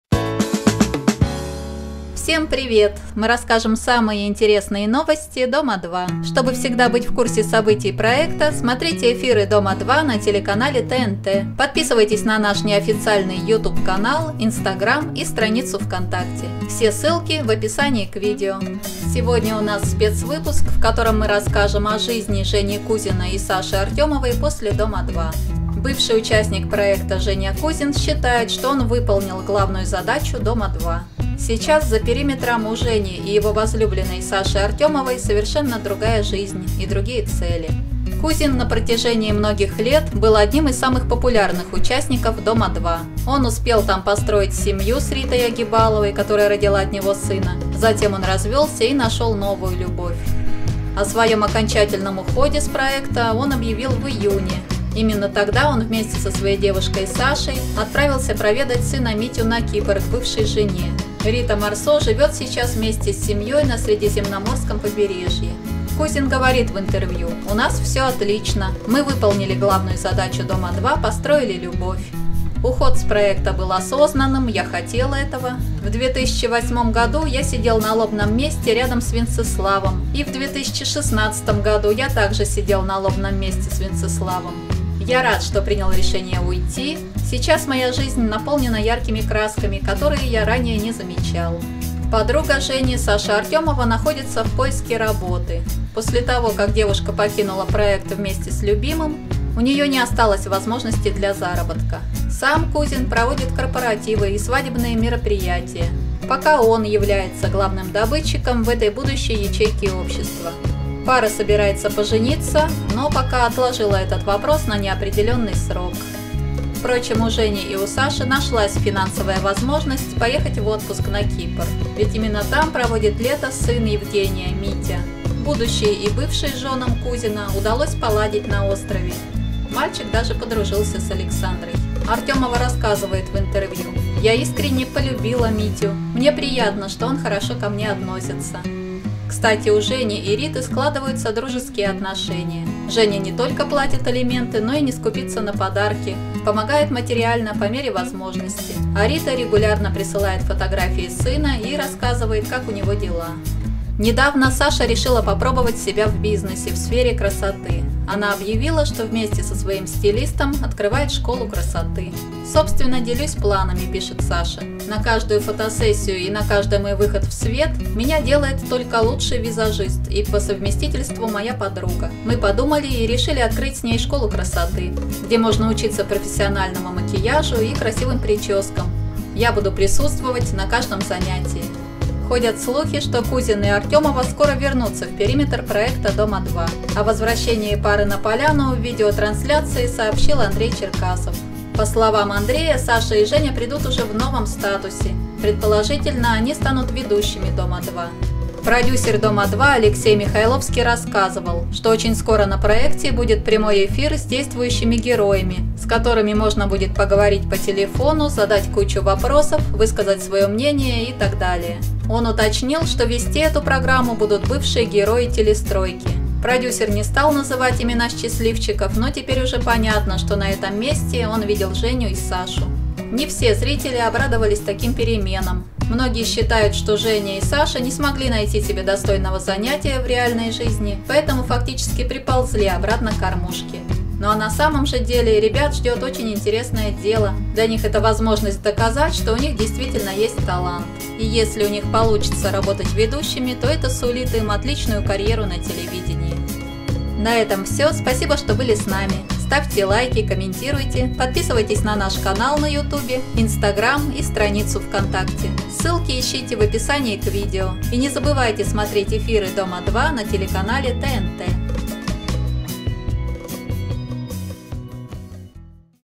СПЕЦВЫПУСК! Евгений Кузин и Саша Артёмова после дома 2! Новости и слухи дома 2.
В этом выпуске новости и слухи  про жизнь Евгения Кузина и Александры Артёмовой после ухода из дома 2.